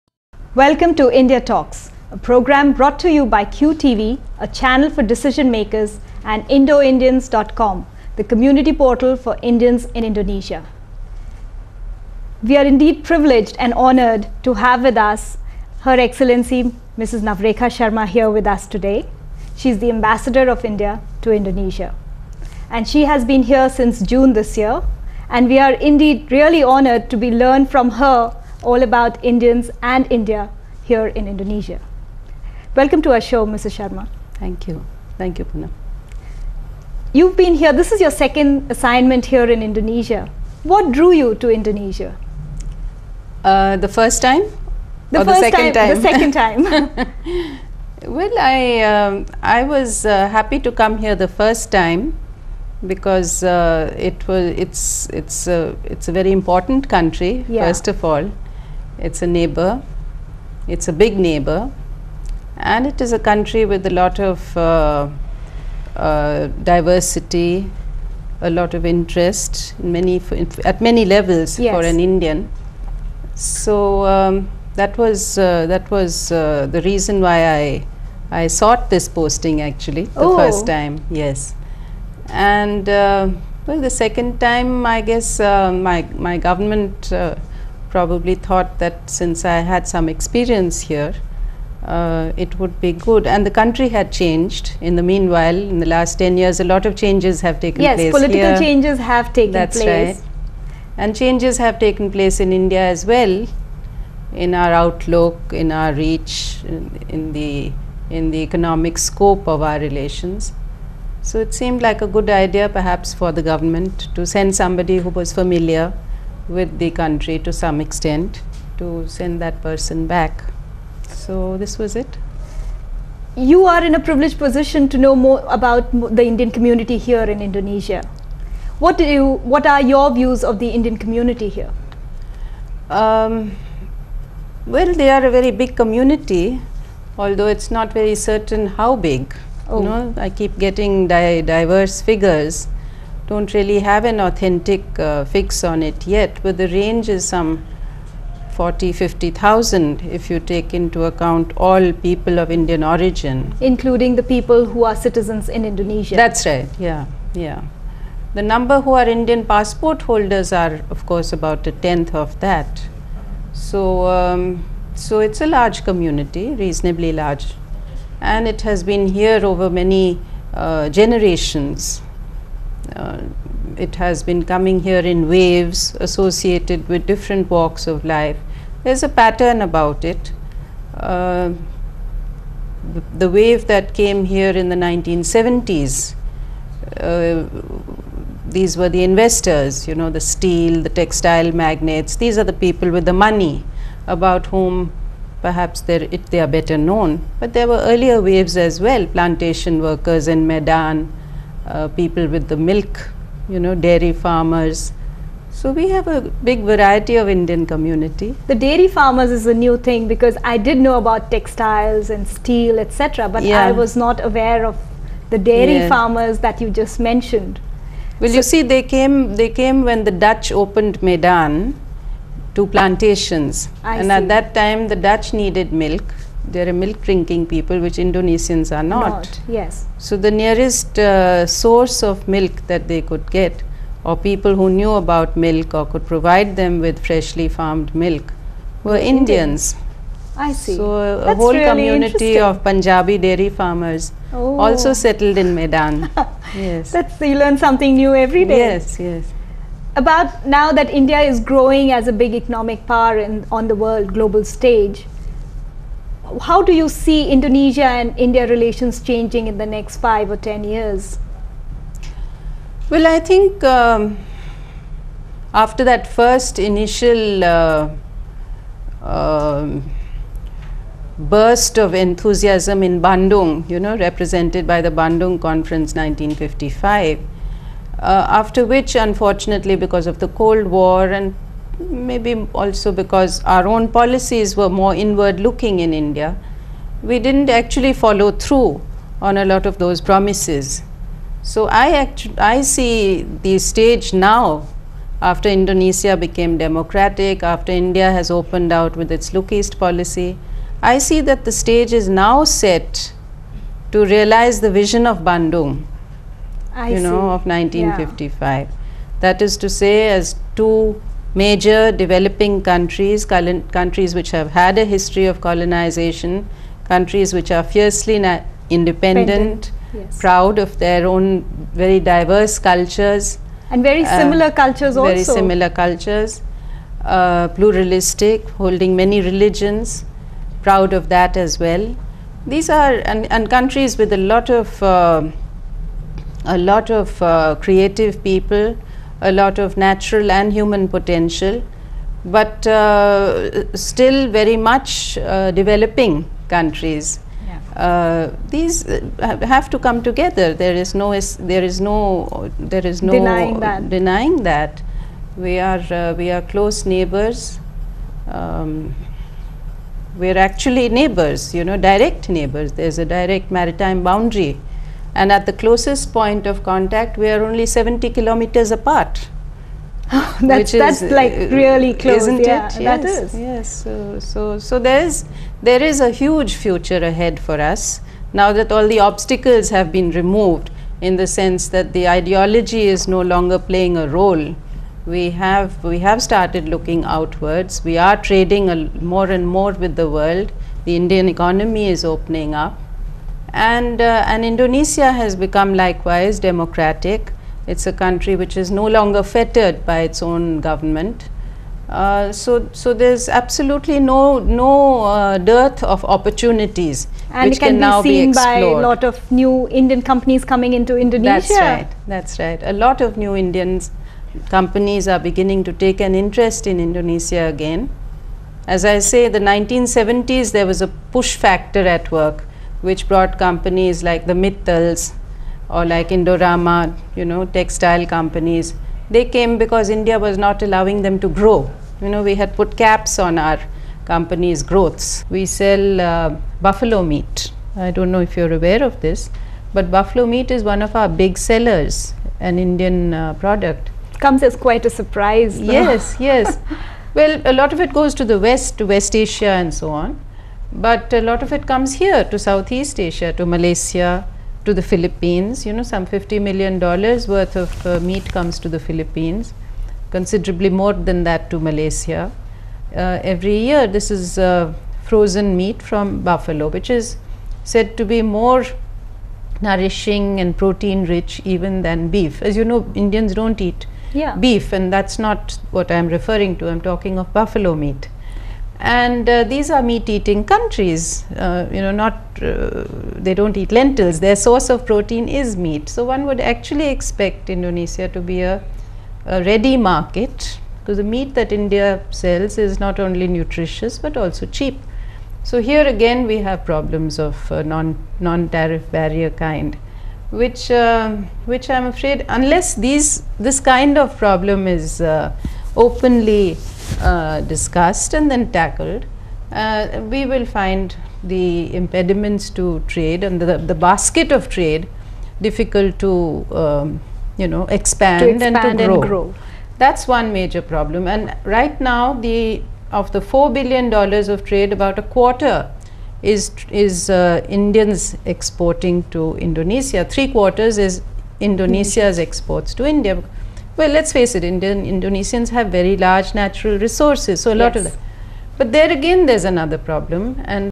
India Talks - Poonam Sagar talks to Navrekha Sharma part 1
Poonam Sagar in conversation with Navrekha Sharma, Indian Ambassador to Indonesia